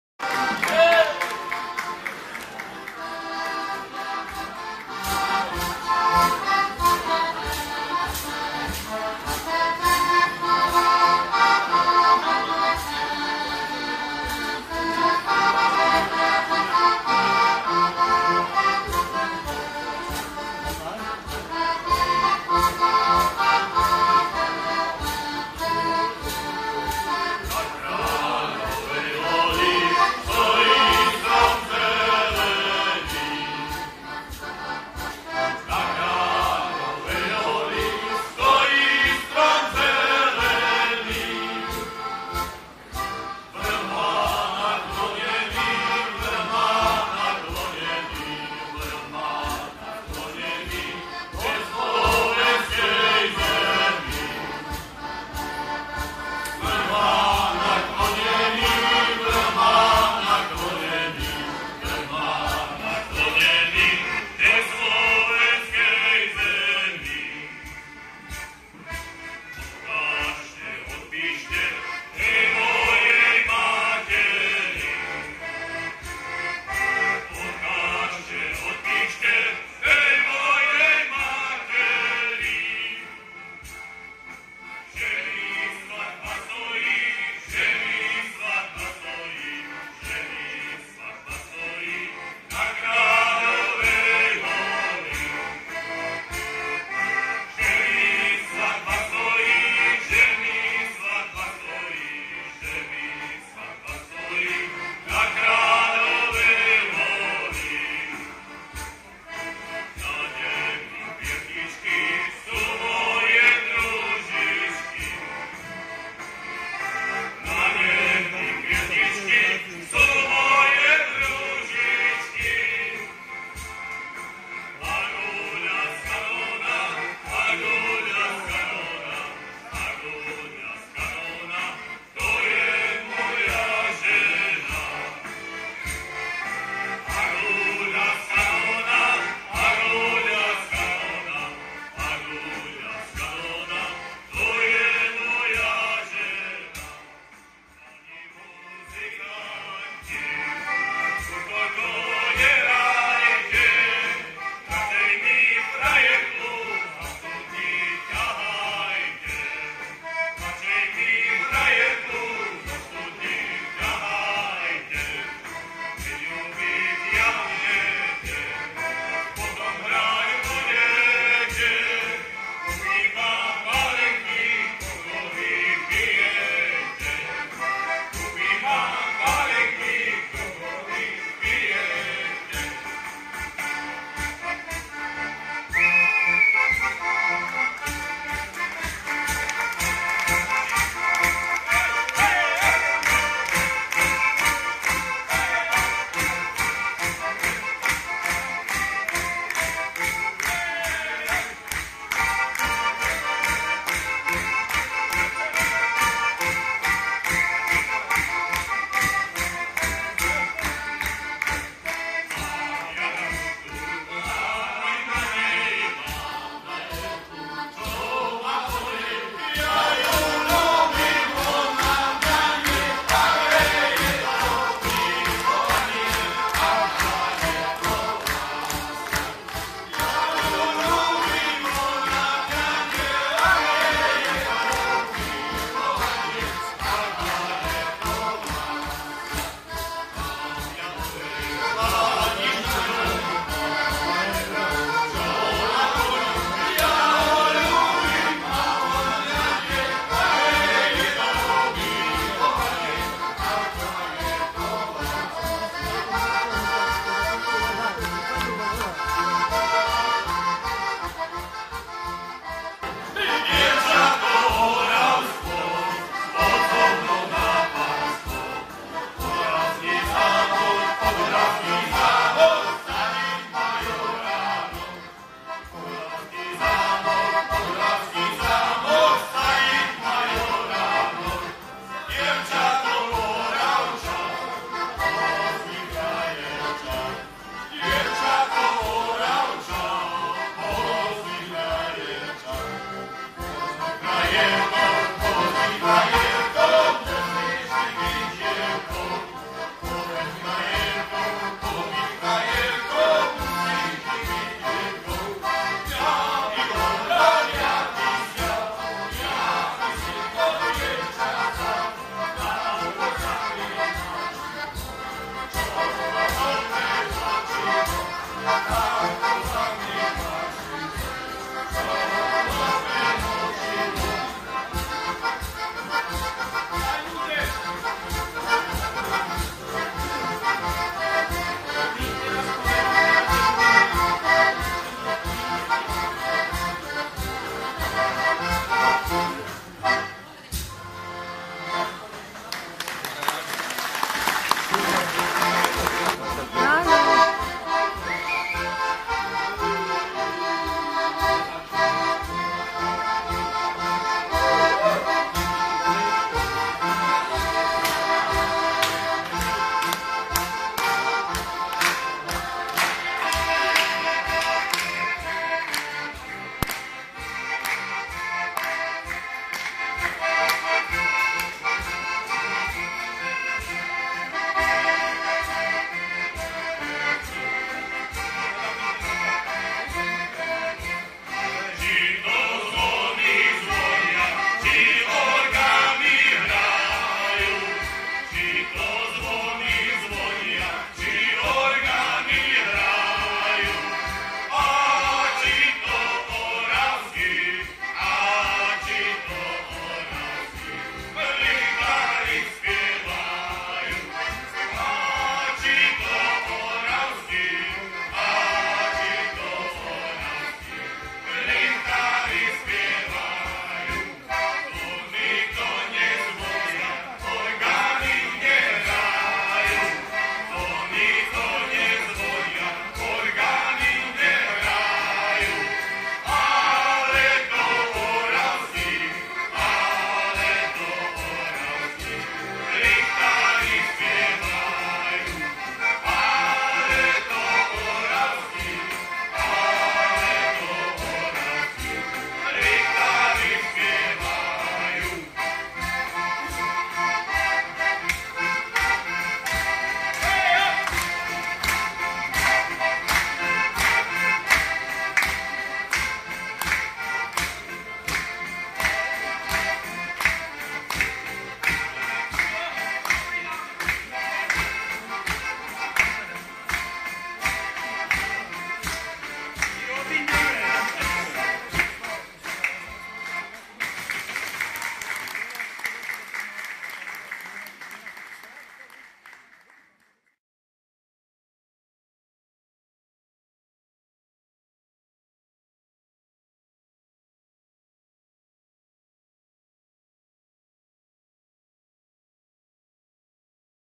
Oravskí richtári na 8. Sajame Slavonski banovac v Pakraci v roku 2016
Oravskí richtári na 8. Sajame Slavonski banovac v Pakraci (Chorvátsko) dňa  15. októbra 2016.

Oravskí richtári je zoskupenie súčasných a bývalých starostov dolnooravských obcí, ktorí spolu hrajú a spievajú od roku 2015. Od začiatku v skupine účinkuje Ján Kamas z Dlhej nad Oravou, Rajmund Bradík z Jasenovej, Jozef Záhora z Oravského Podzámku, Peter Hrivnák z Oravskej Poruby, Metod Sojčák z Pucova, Matúš Mních zo Zázrivej a Milan Pavlovčík zo Žaškova. Neskôr sa k nám pridali aj Zdenko Kubáň z Malatinej a Anton Staš z Chlebníc. Na svojich vystúpeniach spievame väčšinou v sprievode heligónky v rukách Jána Kamasa. Tiež spolupracujeme s ľudovou hudbou súrodencov Murinovcov z Podbiela, so Žaškovskou muzikou a inými zoskupeniami muzikantov.
Pakracké podujatie slavonski Banovac navštevujeme od roku 2015 takmer pravidelne a okrem vystúpenia sa prezentujeme aj slovenskými gastro špecialitami.